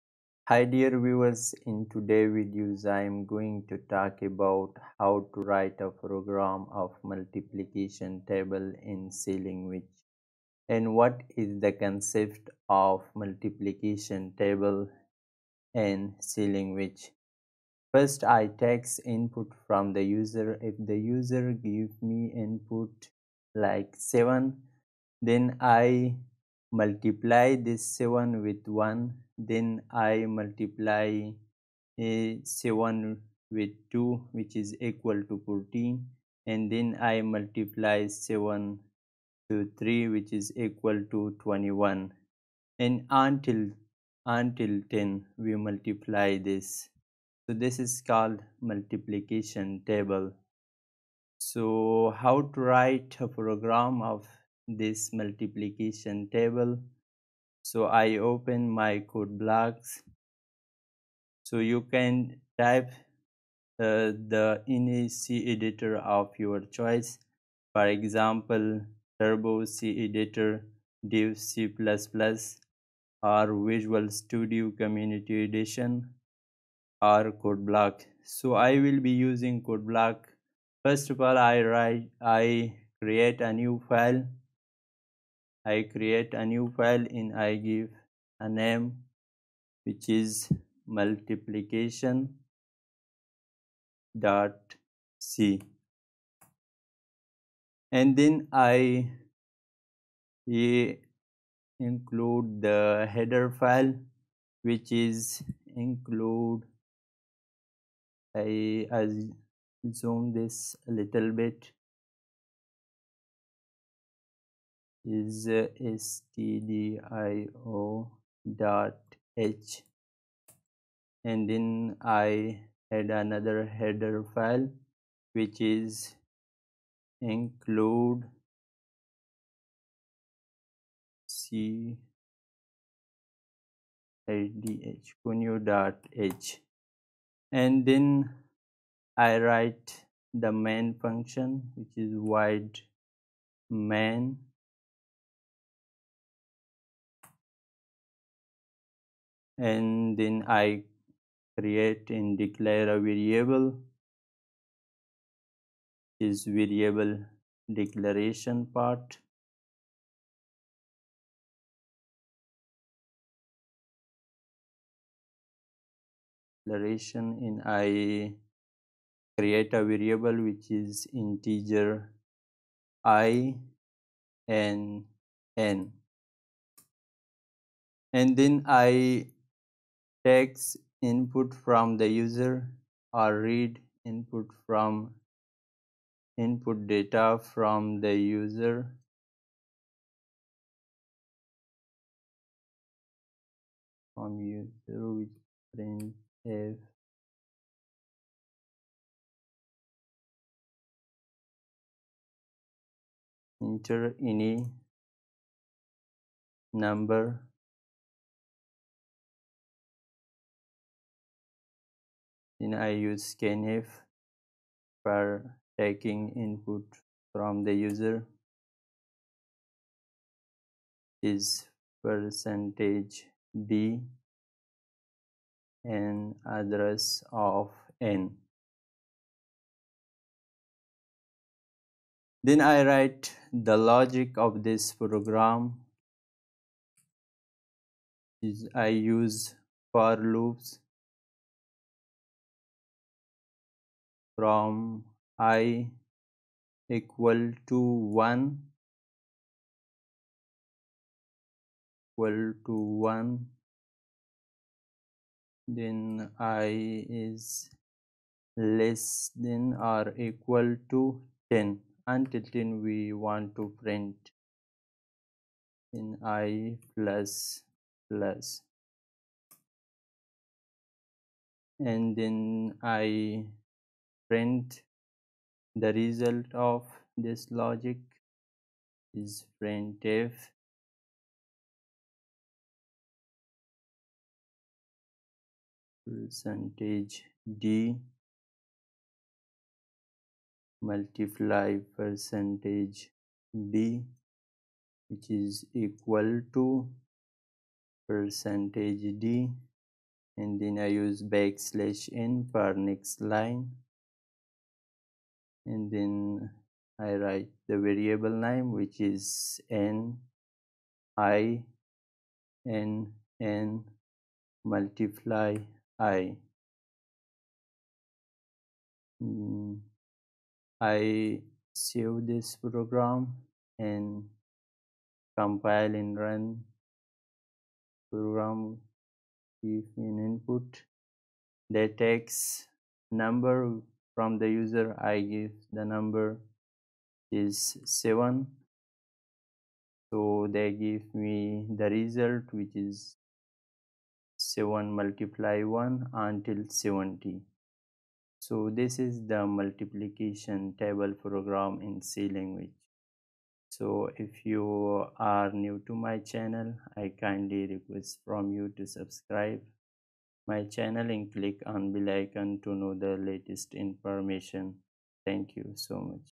C Programs : Print MULTIPLICATION TABLE of given number | CS And SE
In this video, you will learn how to write c program to print multiplication table of a given number.
for Learn programming
Computer science and software engineering